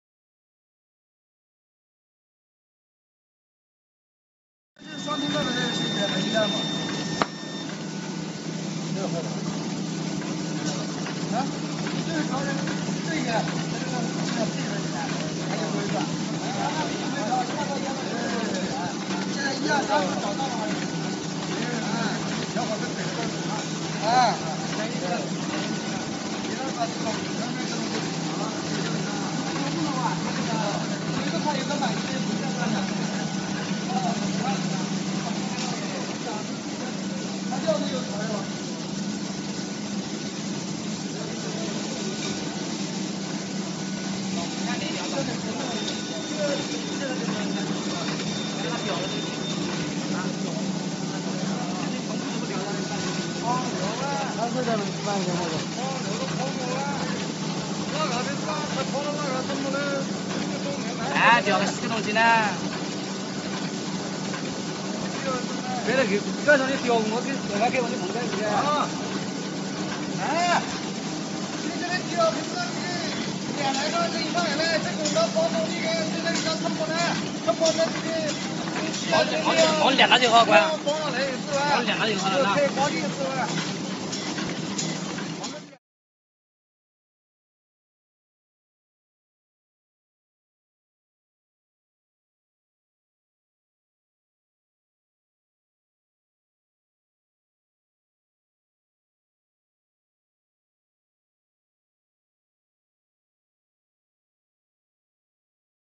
Четырехпоточная машина для производства пакетов GFQ-F700
Предлагаем вашему вниманию видео работы машина для производства пакетов GFQ-F700. С полным описанием и характеристиками вы можете ознакомиться, посетив наш сайт